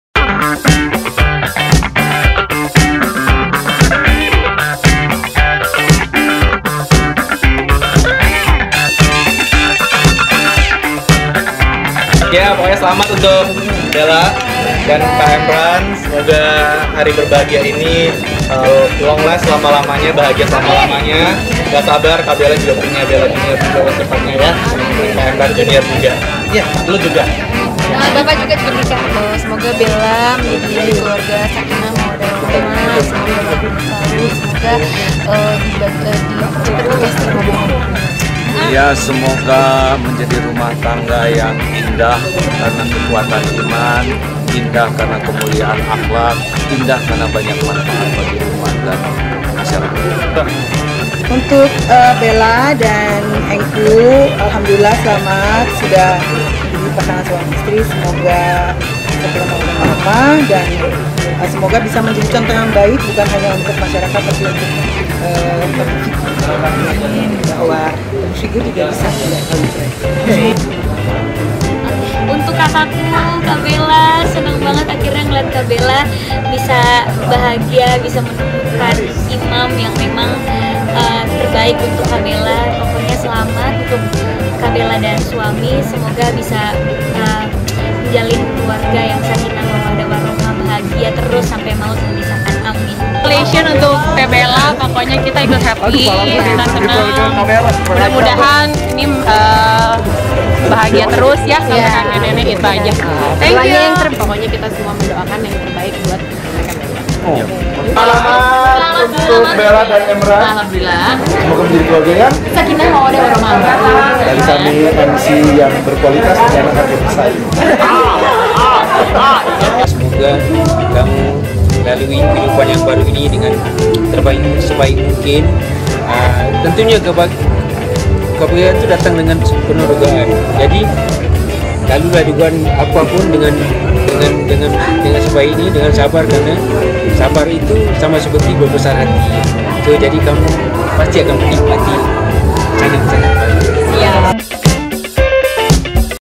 Ucapan Selamat dan Doa Para Artis untuk Laudya Cynthia Bella - Engku Emran
Pada acara resepsi pernikahan Laudya Cynthia Bella dan Engku Emran yang digelar di Bandung akhir pekan lalu, beberapa artis turut hadir dan mengucapkan selamat serta doa untuk pasangan pengantin baru Laudya Cynthia Bella - Engku Emran.

Ini adalah doa dan harapan para artis yang hadir untuk pasangan pengantin baru yang tengah berbahagia, Laudya Cynthia Bella - Engku Emran.

Videografer:  Abdul Rachman
Editor: Idham Harumsyah